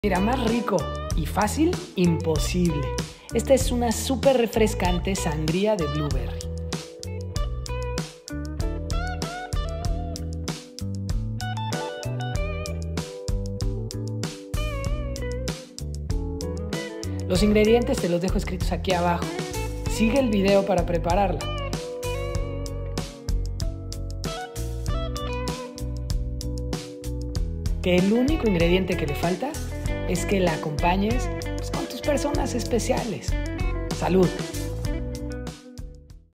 SANGRIA DE MORAS receta súper Fácil y refrescante para el verano
INGREDIENTES SANGRIA DE MORAS
4 tazas de jugo de uva
¼ de taza de arándanos azules
1 taza de zumo de naranja
1 cucharada de zumo de lima
2 naranjas, en rodajas
2 plátanos, en rodajas delgadas
2 limas, en rodajas
1 manzana, en medias lunas
1 melocotón (durazno), en medias lunas
¾ de taza de arándanos azules
4 tazas de hielo
2 tazas agua mineral

¿Todavía no estas inscrito a mi canal de YouTube?

¿Buscas aún más? Entra a